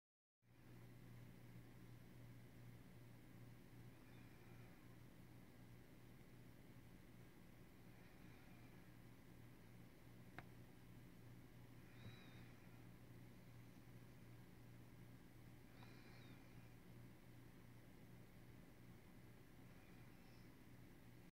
Lice digestion under a microscope - 1 of 4
Ever wondered what it looked like inside a human head louse as it digested your blood?  Here's a video.